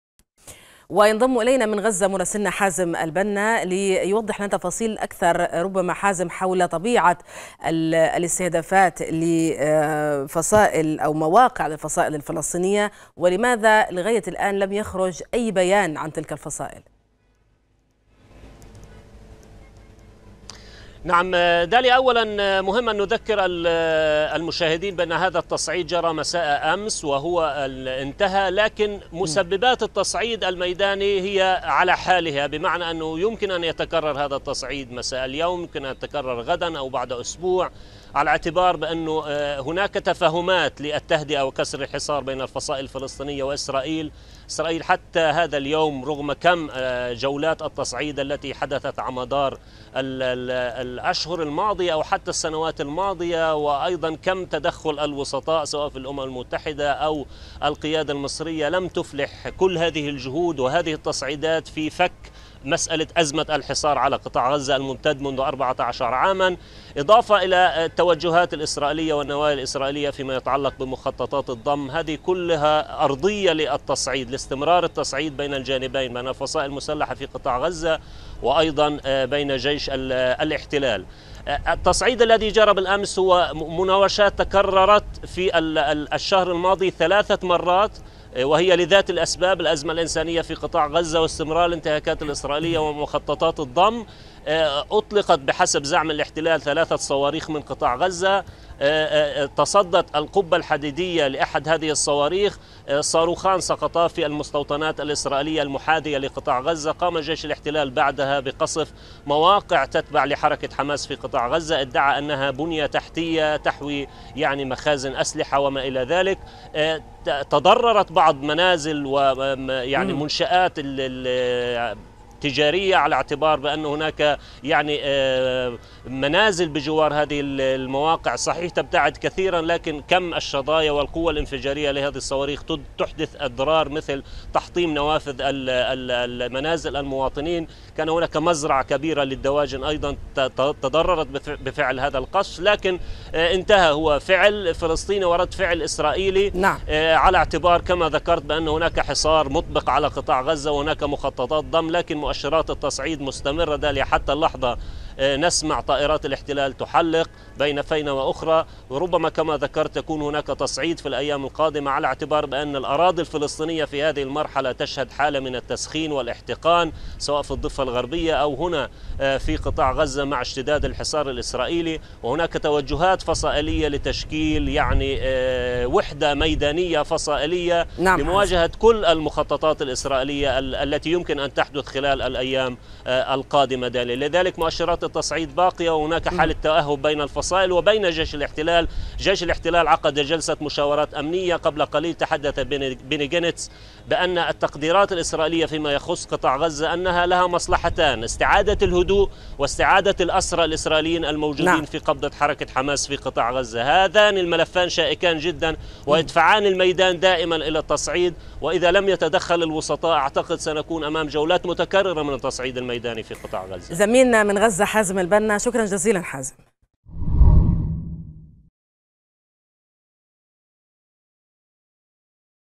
هل تواصل قوات الاحتلال الإسرائيلي تصعيدها بضرب مواقع الفصائل الفلسطينية بغزة؟
أفاد مراسل الغد بأن طائرات الاحتلال الإسرائيلي قصفت مواقع للفصائل الفلسطينية شرق مدينة غزة

 من جانبها ذكرت  وسائل إعلام إسرائيلية أن صافرات الإنذار دوت في المستوطنات القريبة من قطاع غزة وذلك بعد إطلاق قذيفتين من قطاع غزة وقعتا في منطقة مفتوحة دون التبليغ عن وقوع إصابات.

المزيد من التفاصيل مع مراسل الغد حازم البنا.